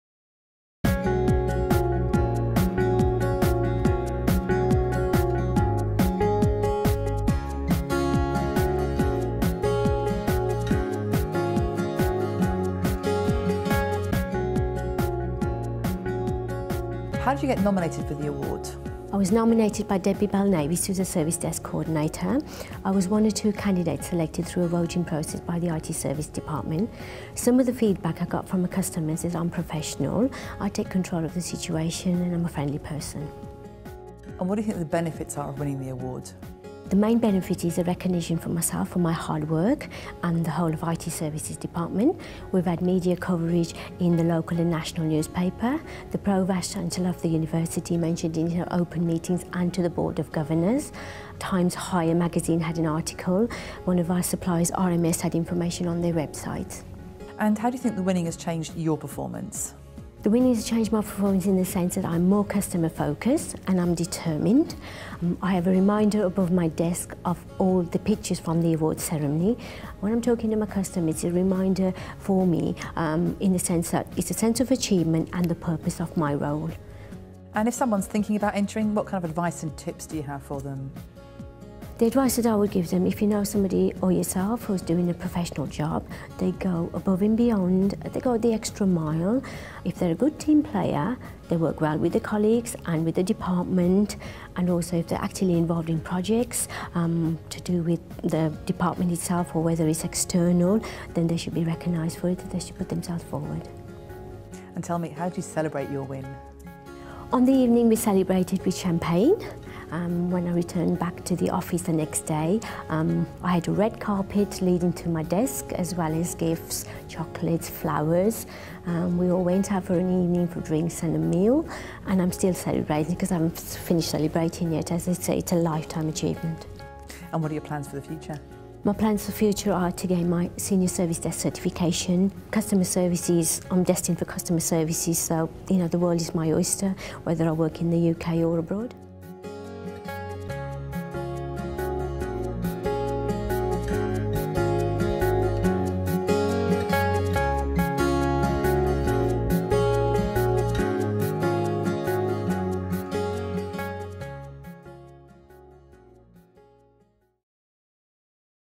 Winner of the IT Service Professional of the Year Award 2009 - Nalini Patel
Chantal Cooke interviews Nalini Patel - Winner of the IT Service Professional of the Year  Award 2009